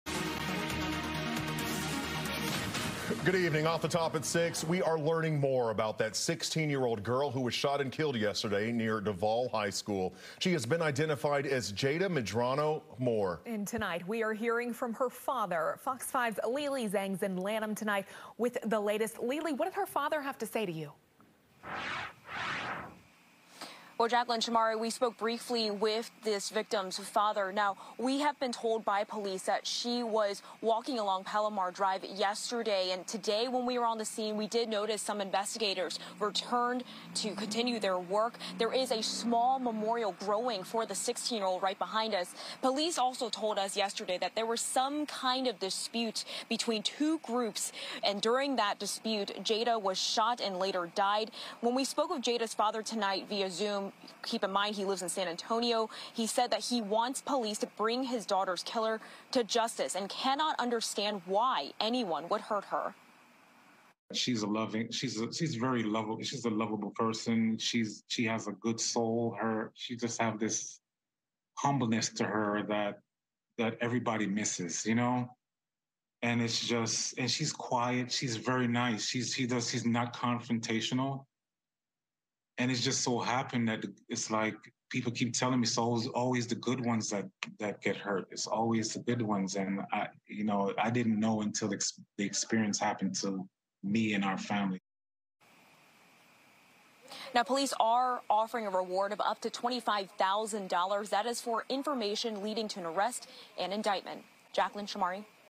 Greenbelt teen shot, killed walking home from school
The 16-year-old girl who was shot and killed near her high school in Lanham Monday has been identified as Jayda Medrano-Moore. Police are still looking for the shooter. FOX 5's Lili Zheng reports from Prince George's County with the latest details.